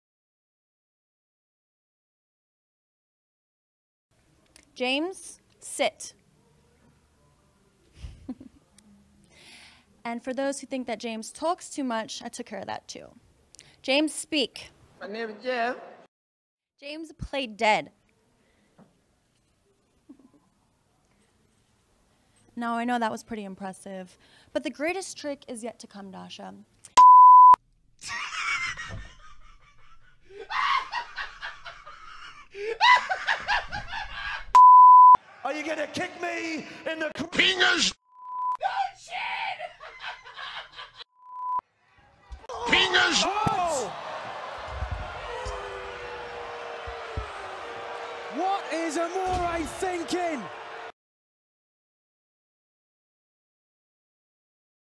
WWE crack 1 - feat. Keemstar laughing, Jeff Ellsworth, Enzo kicks PINGAS
In this WWE crack, Keemstar laughs as he reveals FaZe Banks shares a finisher with Sasha Banks, Carmella has a pet Ellsworth and his name is Jeff, and Enzo Amore kicks Neville in the PINGAS!